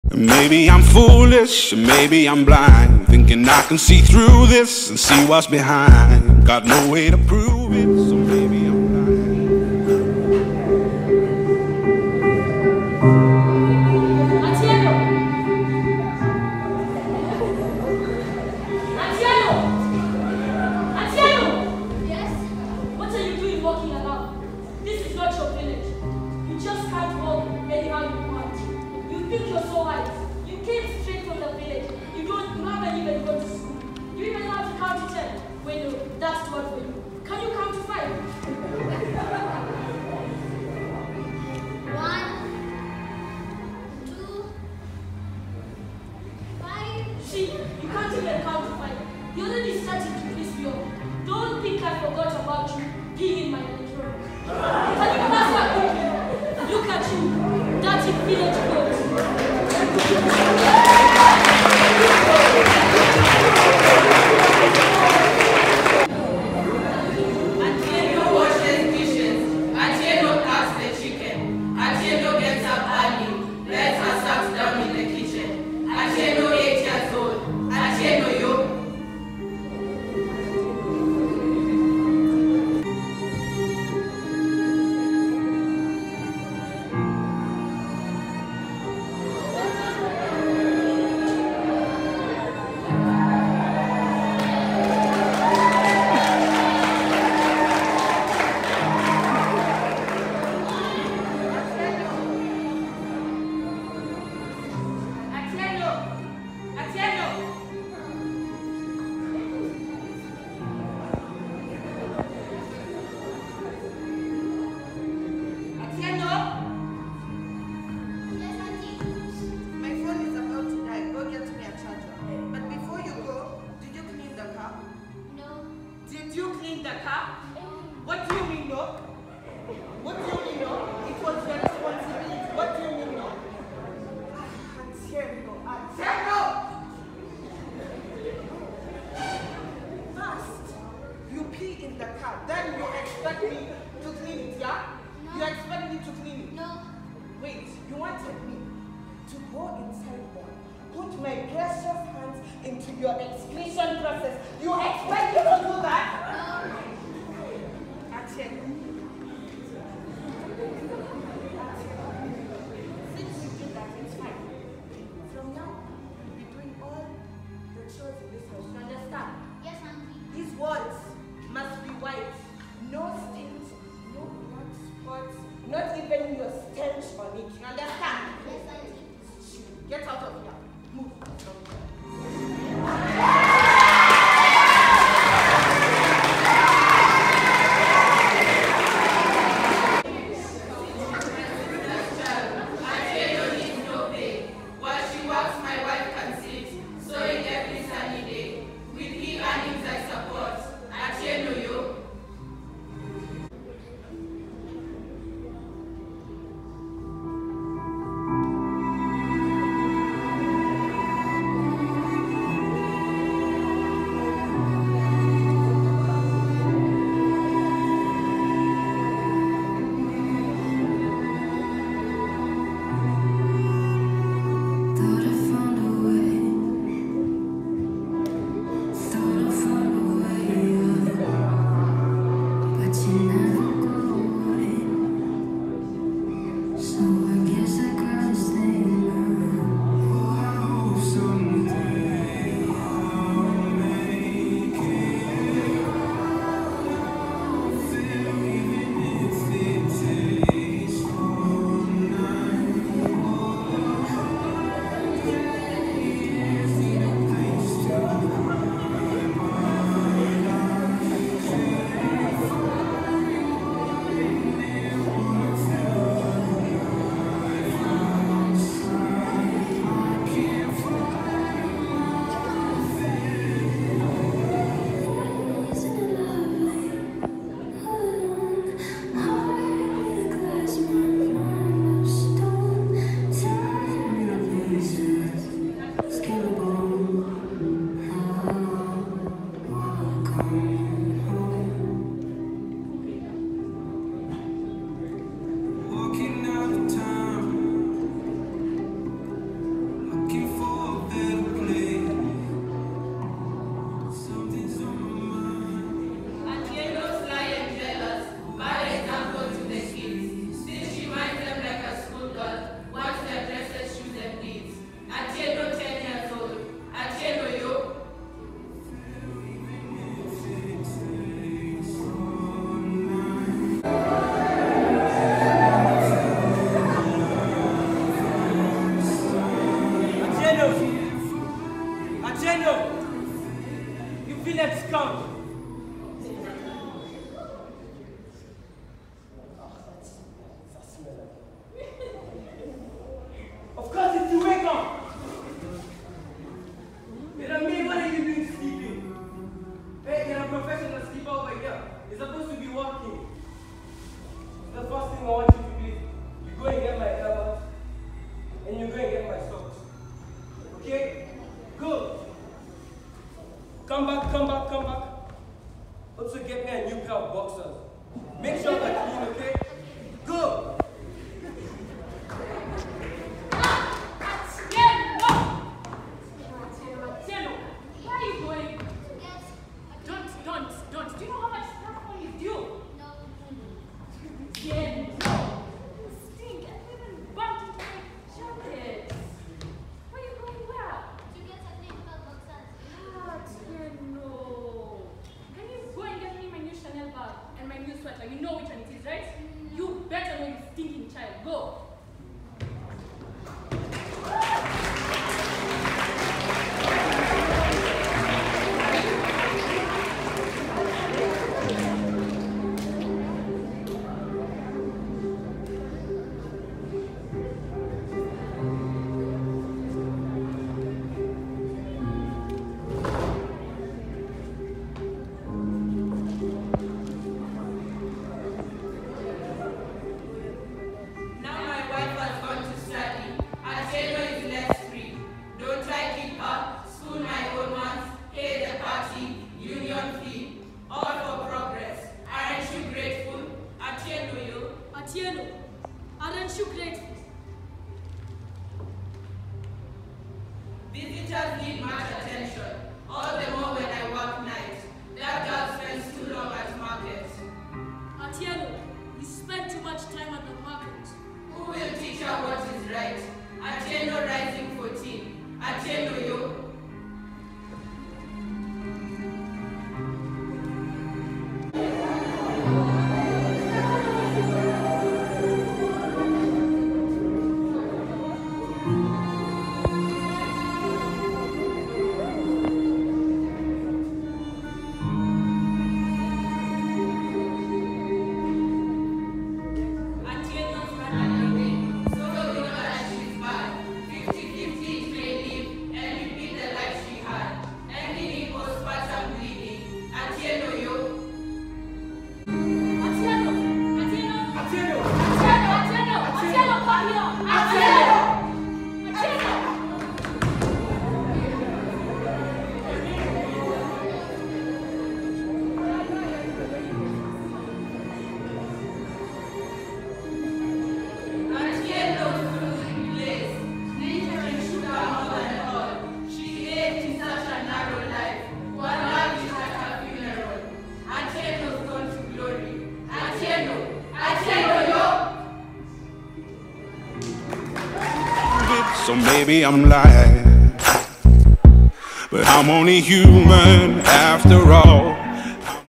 A Freedom Song- Marjorie Oludhe Macgoye
'A freedom song' is a poem by Marjorie Oludhe Macgoye. We adapted it into a skit accompanied by a choral rendition for our Year 9 Assembly.
The poem is about a young girl called Atieno who is mistreated by her own maternal uncle. She works without pay, she is not sent to school, she lacks parental guidance thus she gets early pregnancy as a result she ends in death due to post-partum bleeding.

Through this poem, we are advocating for the need to treat others right and not taking advantage of their situation.

St. Austin's Academy